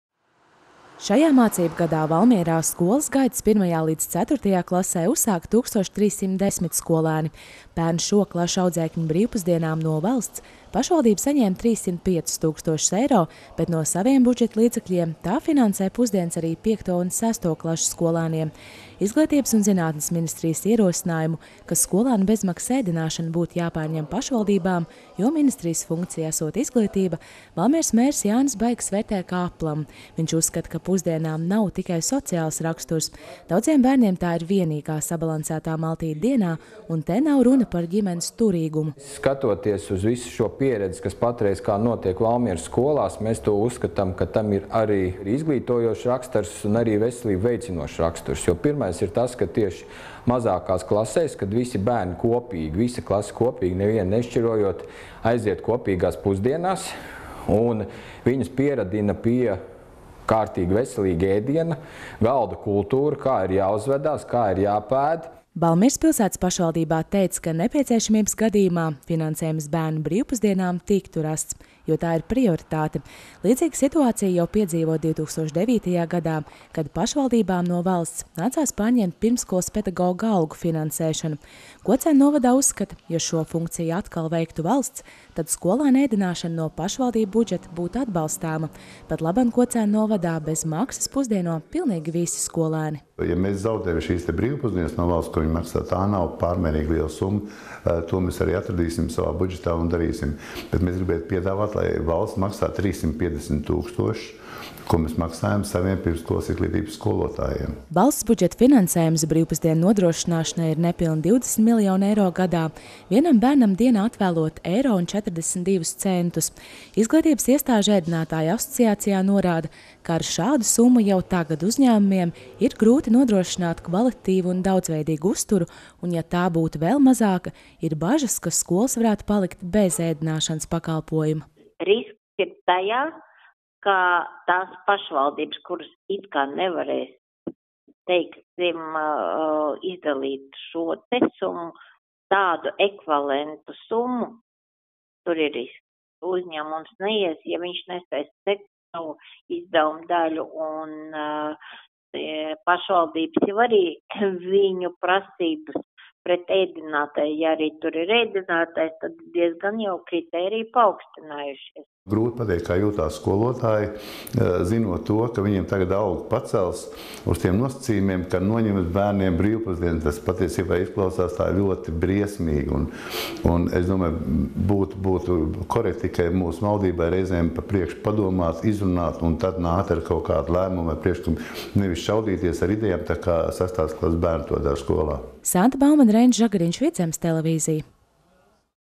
Brīvpusdienas nodrošina 1.-6.klašu skolēniem
Nesenais Izglītības un zinātnes ministrijas priekšlikums uzticēt 1.-4.klašu skolēnu brīvpusdienu finansēšanu pašvaldībām radījis vecāku jautājumus par bērnu bezmaksas ēdināšanu jaunajā mācību gadā. Valmieras pilsētas pašvaldība informē, ka, neskatoties uz iespējamo finansējuma pārdales maiņu nākotnē, pašvaldības dibināto izglītības iestāžu 1.-6.klašu skolēniem tiek garantēta brīvpusdienu saņemšana viņu apmeklētajā mācību iestādē.